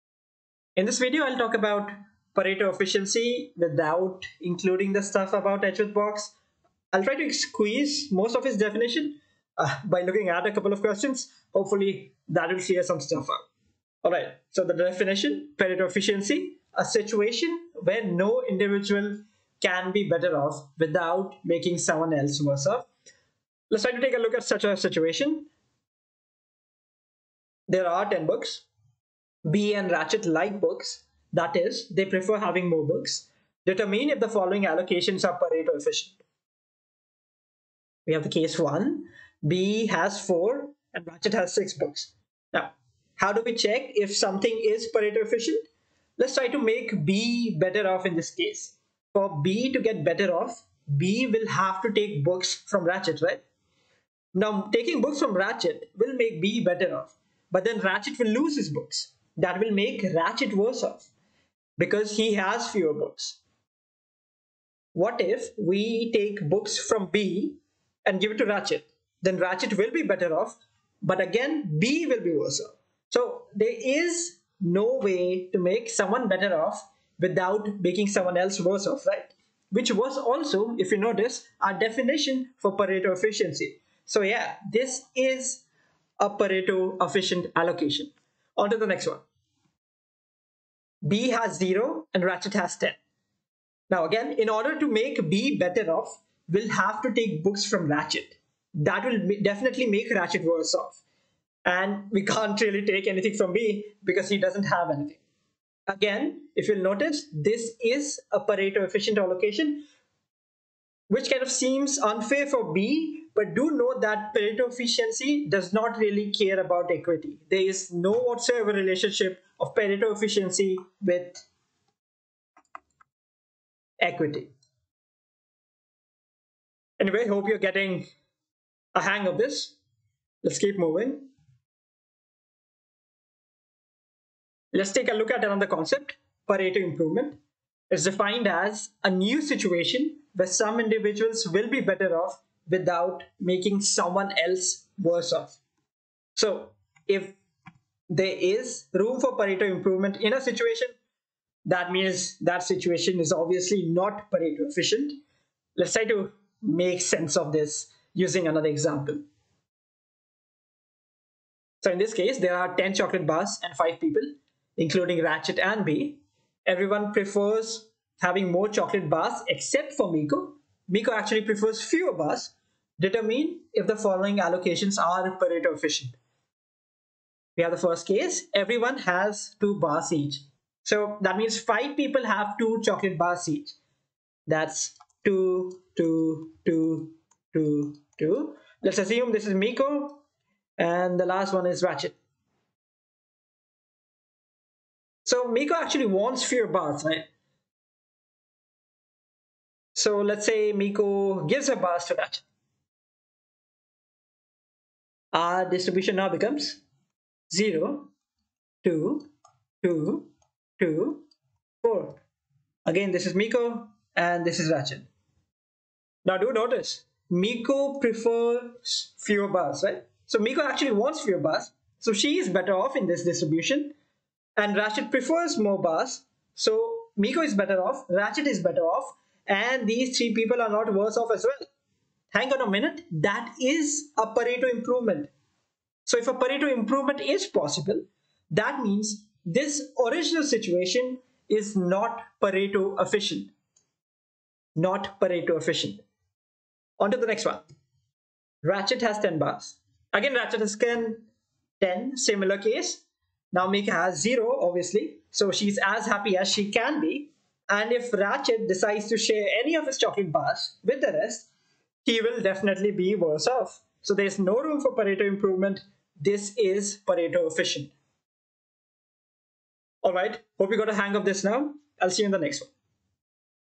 Pareto Efficiency: Explained using examples
Pareto Efficiency explained using a couple of examples.